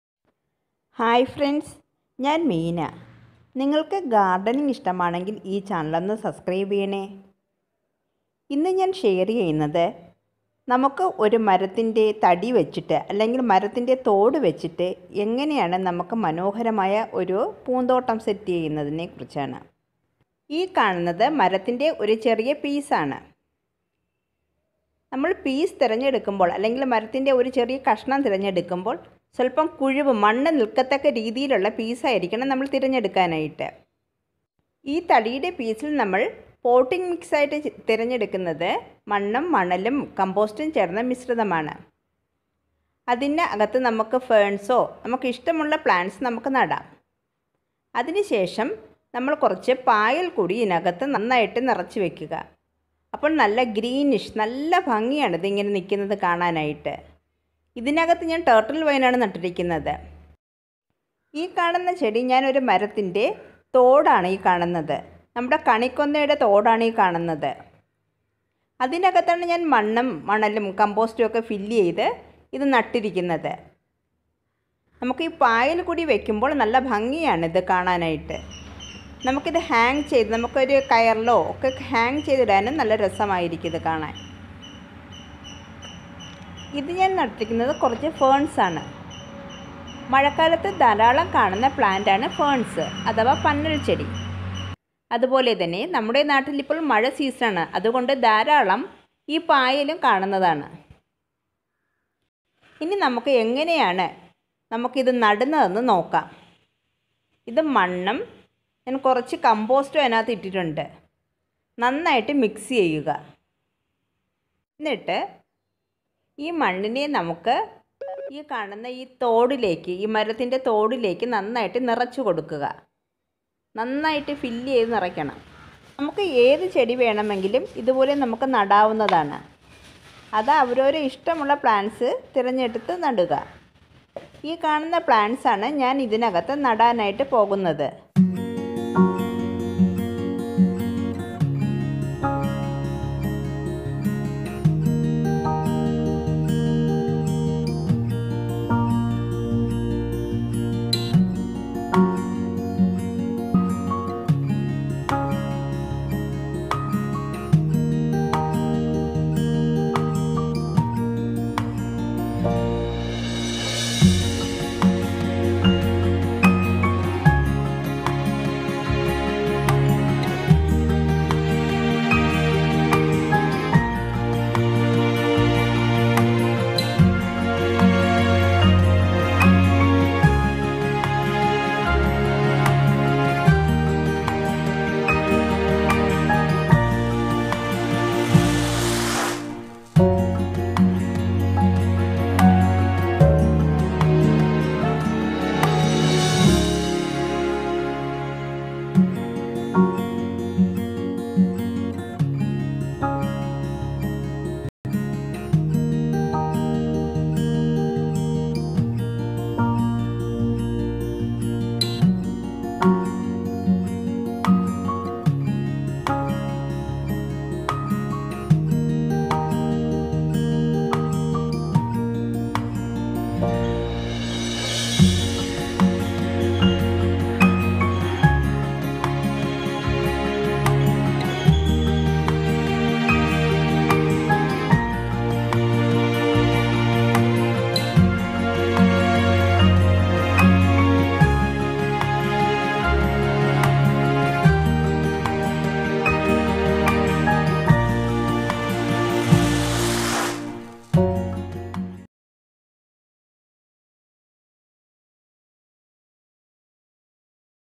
DIY wooden bark garden#mossgarden#പാഴ്ത്തടിലെ പൂന്തേട്ടം
This video is about some gardening ideas using bark of a tree.